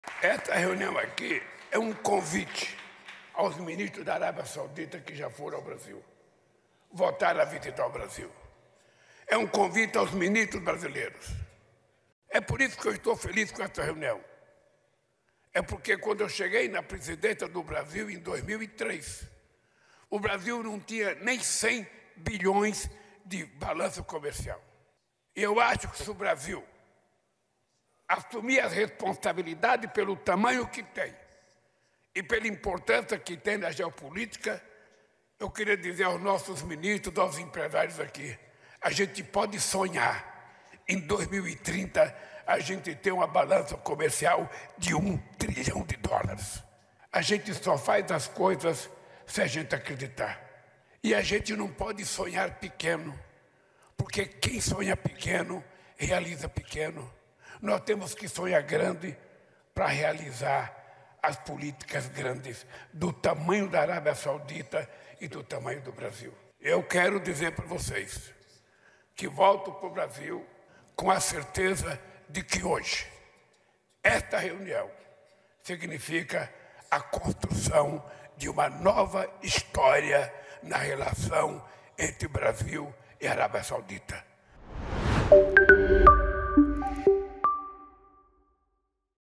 Uma nova história nas relações Brasil e Arábia Saudita
Estamos construindo uma nova história nas relações entre Brasil e Arábia Saudita. Nossa parceria voltará a crescer na esfera bilateral e vai se expandir nos foros multilaterais para garantir novos investimentos e desenvolvimento sustentável.
🎥 Audiovisual/ PR